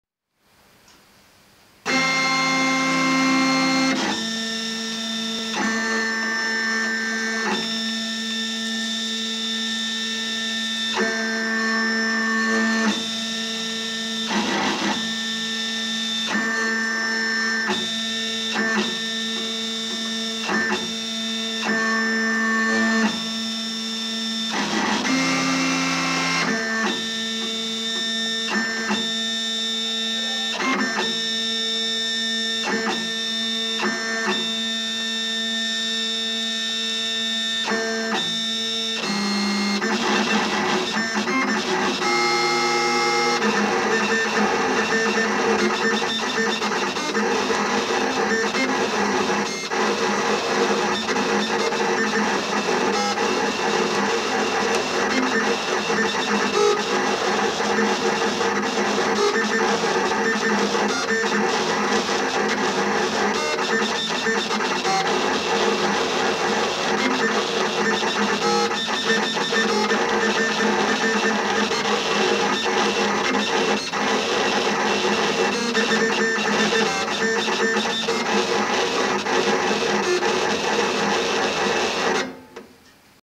Cutting plotter drawing the "Resistance is futile" graphic with a pen
We put a pen into our cutting plotter and had it draw the "Resistance is futile" graphic. This is how a cutting plotter works!